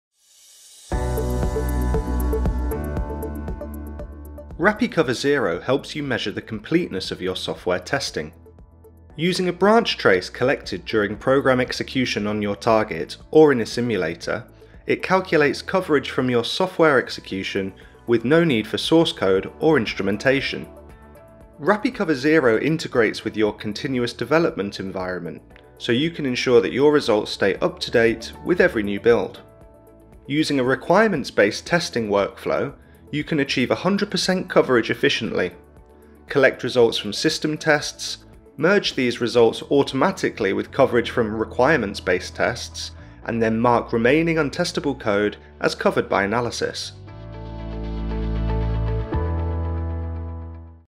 Zero footprint coverage analysis with RapiCover Zero
RapiCover Zero lets safety-critical software engineers calculate the structural coverage achieved by their tests with no need for instrumentation or access to source code.

Its justifications let you mark untestable code as covered by analysis, while you can also merge coverage from different test runs, even when your code changes between them.